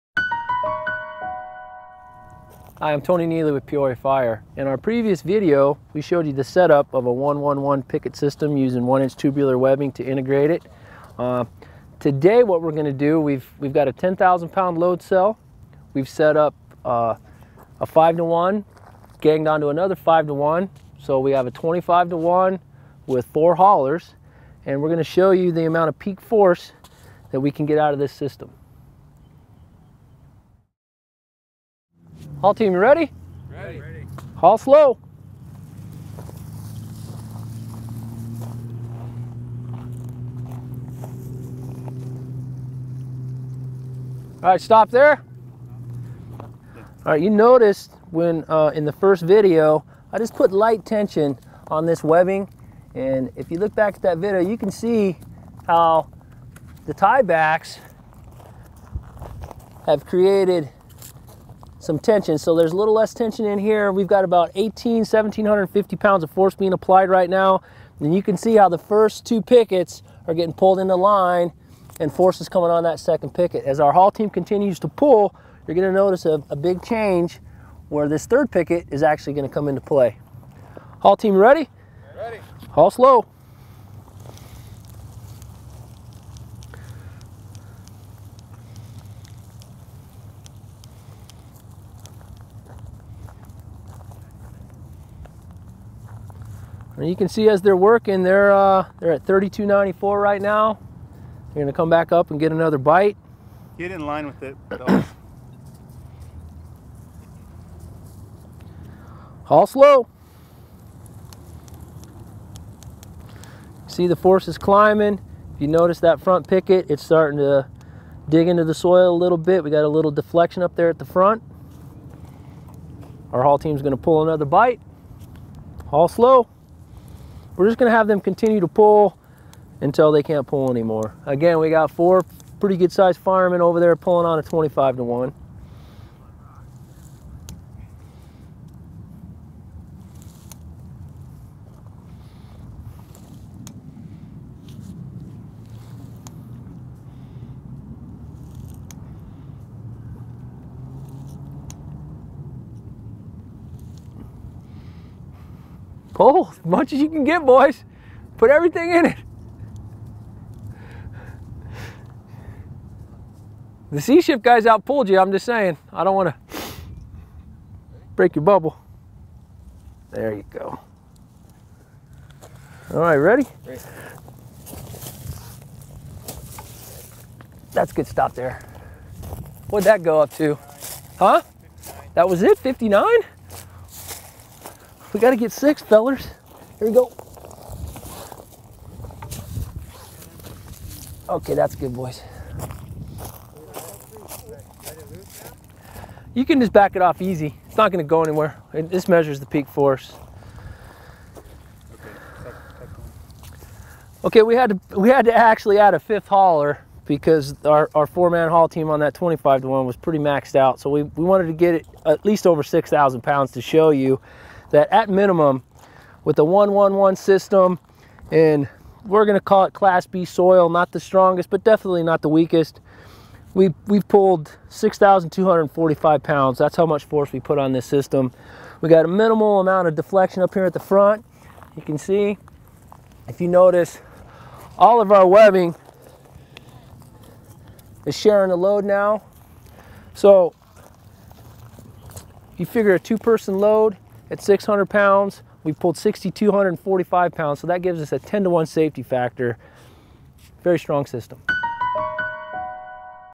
Picket Anchor Pull Test
How strong is a Piclet anchor system? DRR and Peoria Fire pull a 1-1-1 Picket system with a 25:1 MA. The solid is dry type B soil. Watch this for some confidence in your picket system.